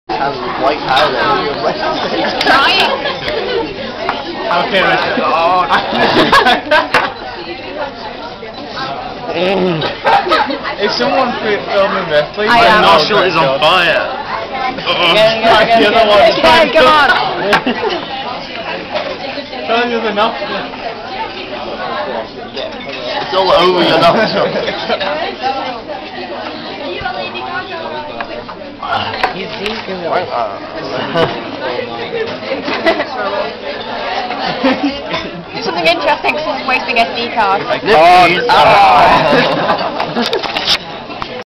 The Beverage Snorts Pepper
So this is what we did when we went to Paris. It was out of focus on the camera, so the quality's terrible.
Paris Trip 2009